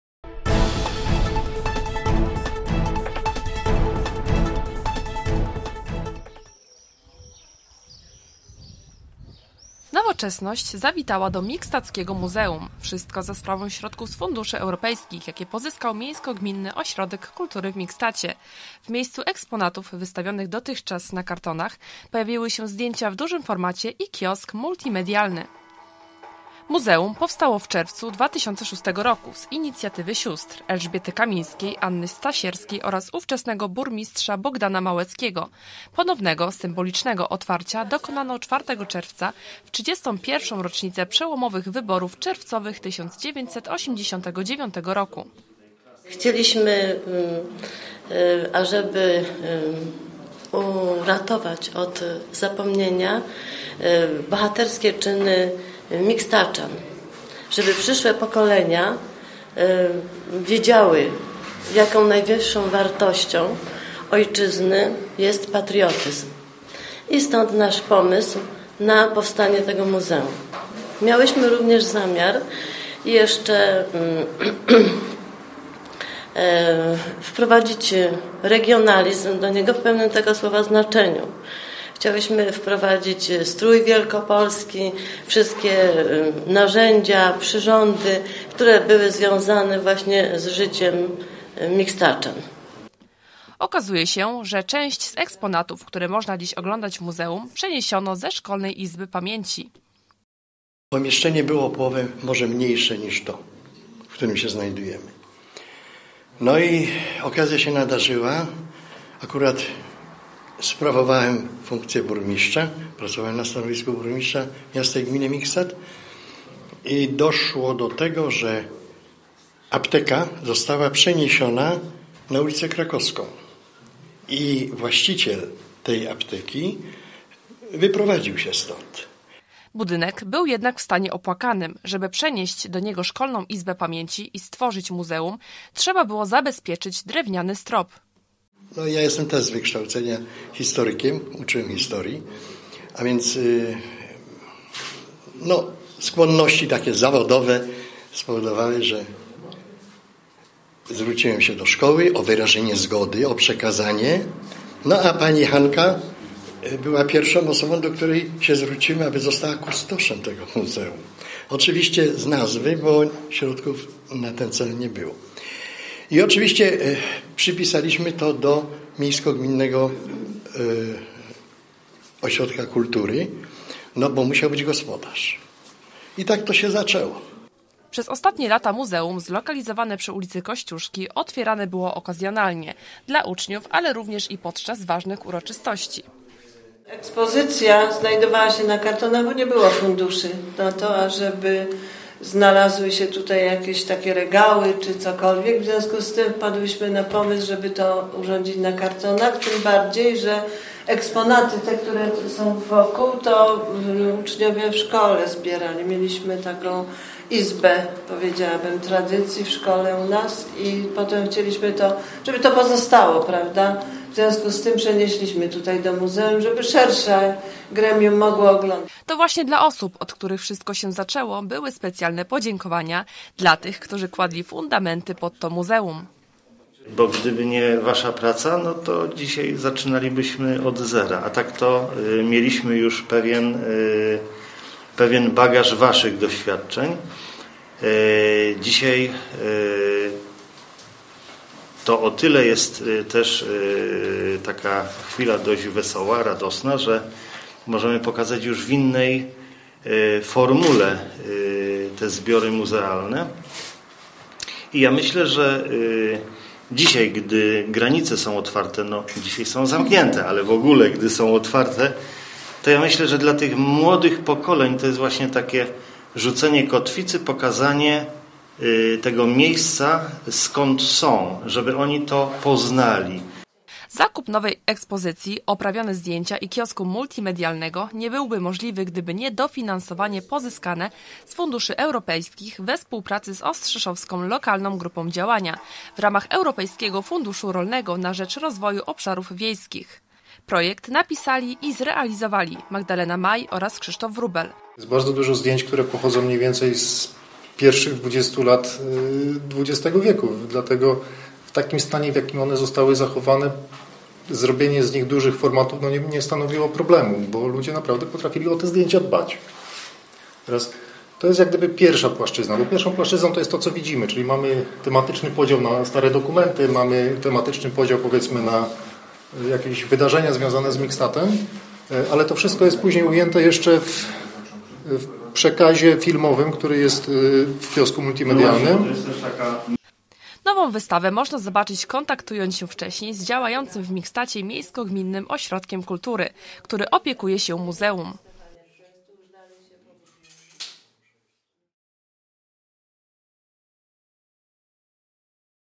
Nowoczesność zawitała do mikstackiego muzeum
Nowoczesność zawitała do mikstackiego muzeum. Wszystko za sprawą  środków z funduszy europejskich, jakie pozyskał Miejsko-Gminny Ośrodek Kultury w Mikstacie. W miejscu eksponatów wystawionych dotychczas na kartonach, pojawiły się zdjęcia w dużym formacie  i kiosk multimedialny.
Muzeum powstało w czerwcu 2006 roku z inicjatywy sióstr: Elżbiety Kamińskiej, Anny Stasierskiej oraz ówczesnego burmistrza Bogdana Małeckiego. Ponoownego, symbolicznego otwarcia  dokonano 4 czerwca w 31. rocznicę przełomowych wyborów czerwcowych 1989 roku.